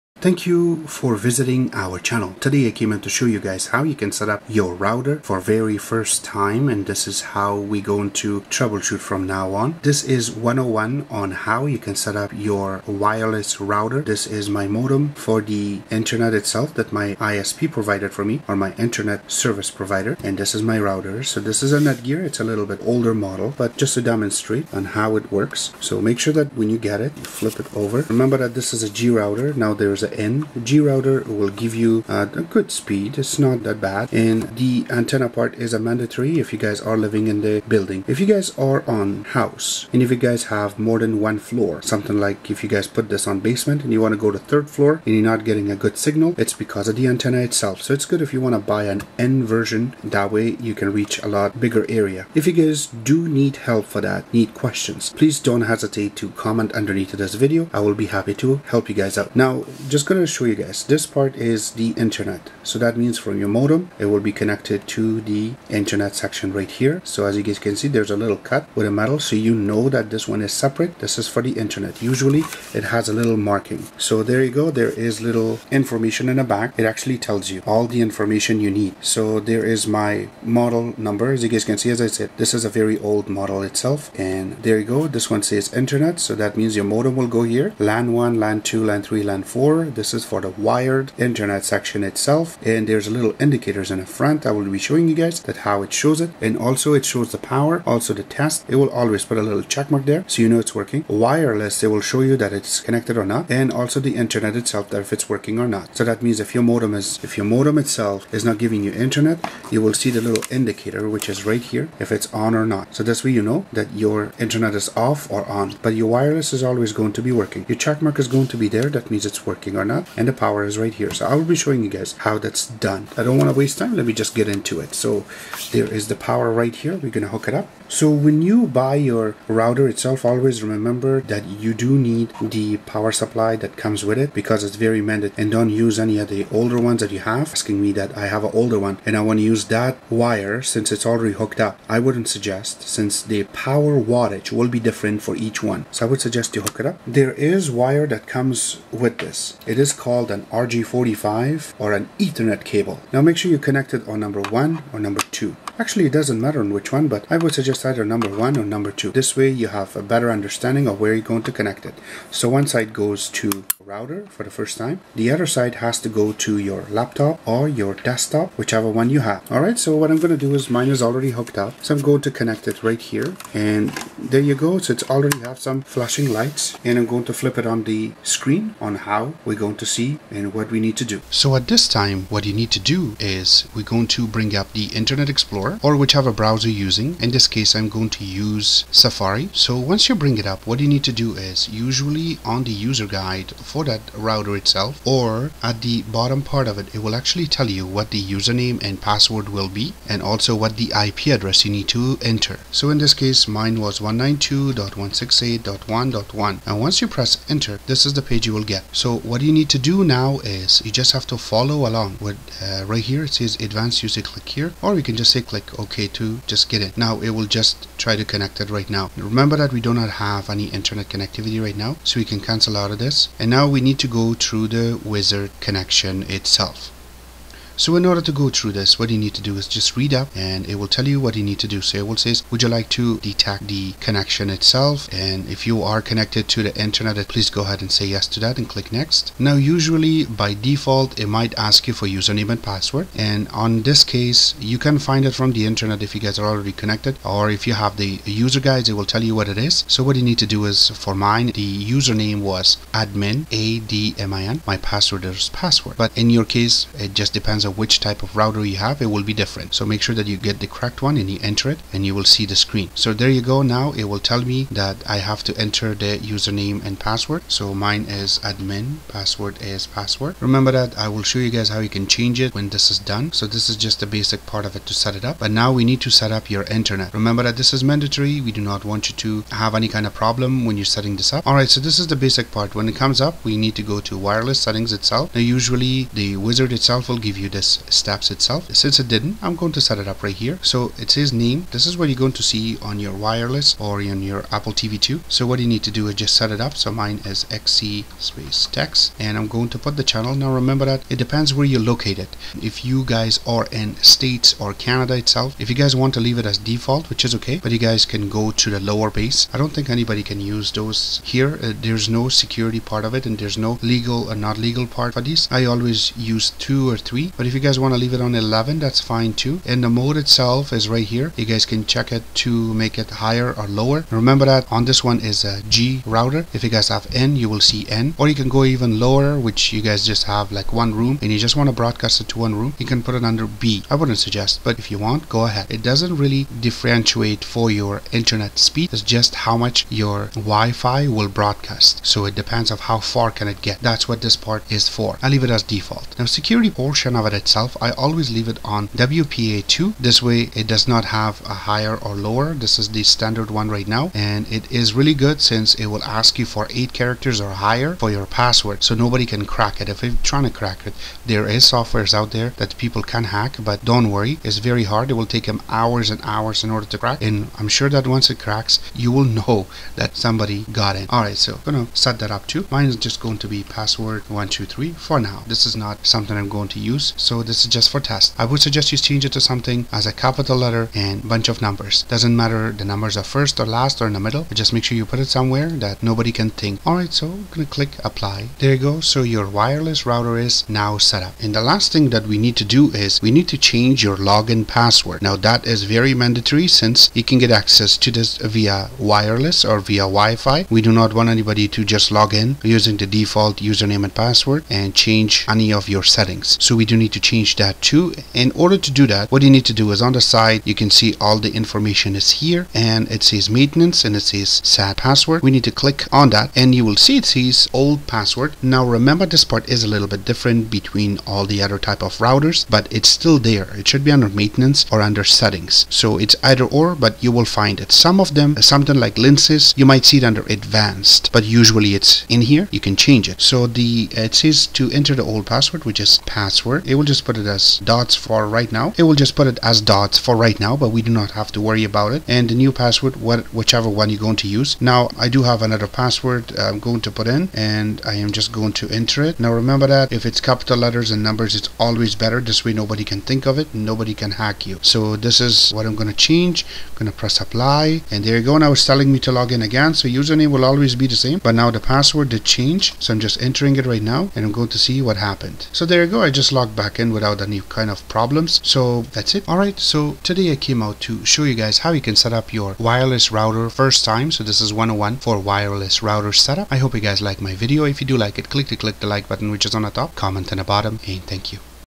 Router setup 101 - How To - Series 1
Since alot of you, kept asking me about how did i setup my router and what i meant by same router I thought i should start a few videos as series that way i could tell you to watch series 1 or 2 or 3 or more to figure out what has happened or what your doing is wrong,

But to setup Router, here is basic setup video